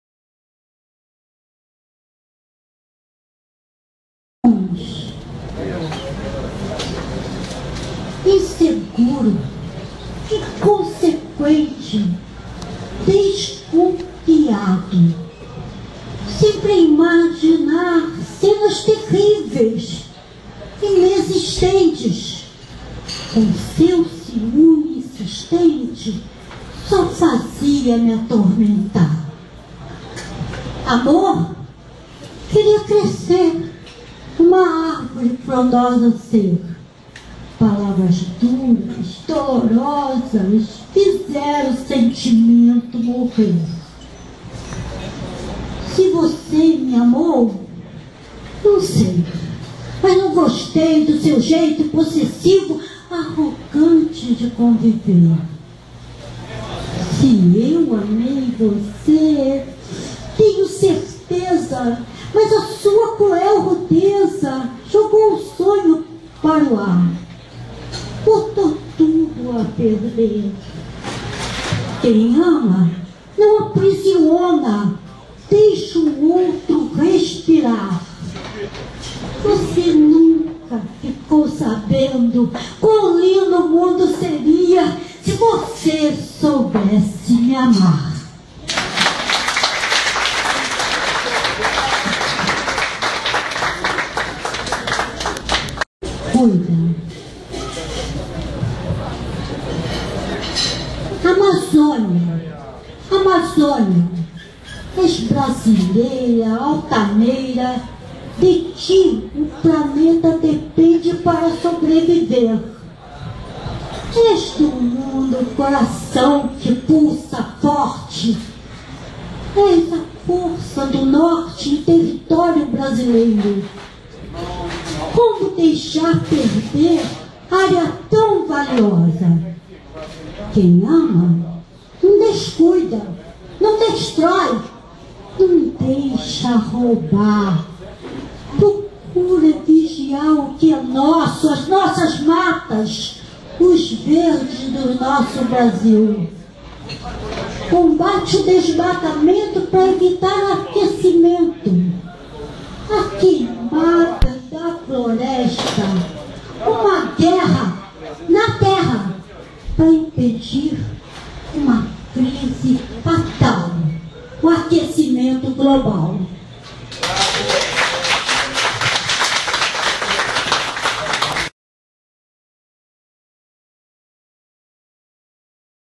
Dionilce Silva de Faria no Corujão da Poesia em Niterói
Dionilce Silva de Faria, tecnóloga Educacional, pesquisadora, autora da gramática É Fácil Aprender Português! participou do Corujão da Poesia em Niterói e declamou duas poesias de sua autoria: "Ciúmes" e "Amazônia".